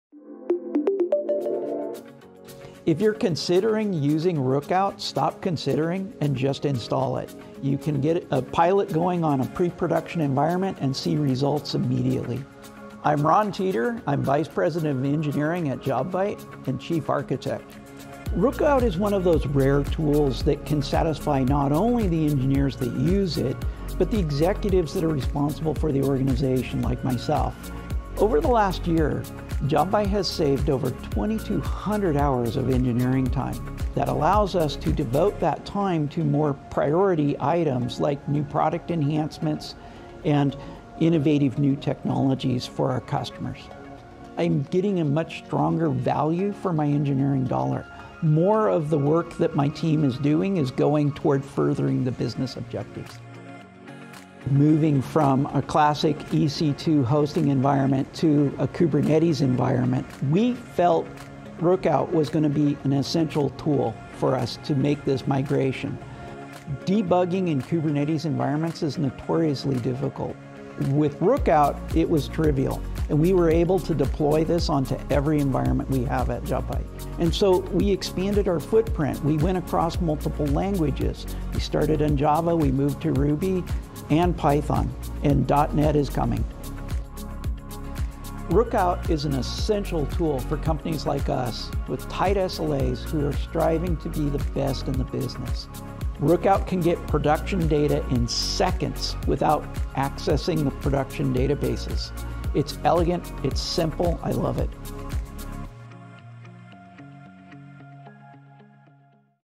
Rookout and Jobvite Case Study Testimonial
Ron Teeter, VP of Engineering and Chief Architect at Jobvite, discusses why their R&D team loves using Rookout and how it saves them hours of work and frustration.